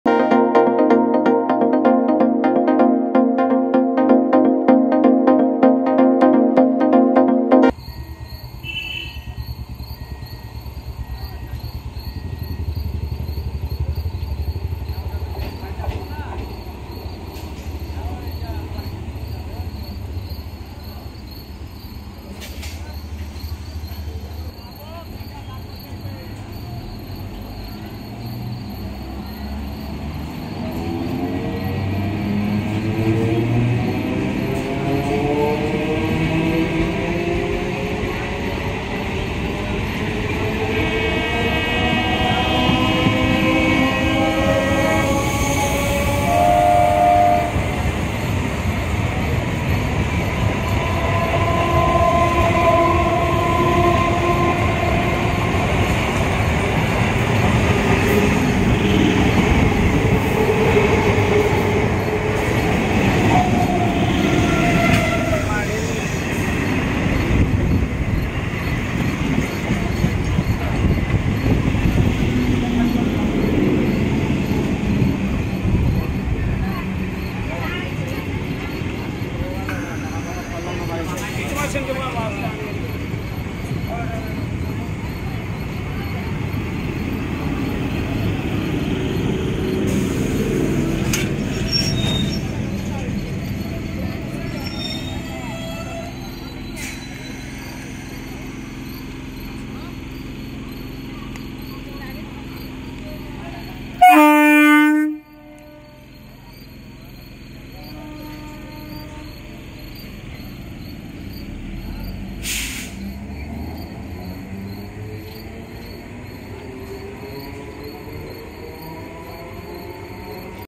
Amazing and exciting crossing at night between modern BEML and old EMU train
Amazing and exciting crossing at night between modern BEML and old EMU train. I am publishing YouTube shorts on my channel.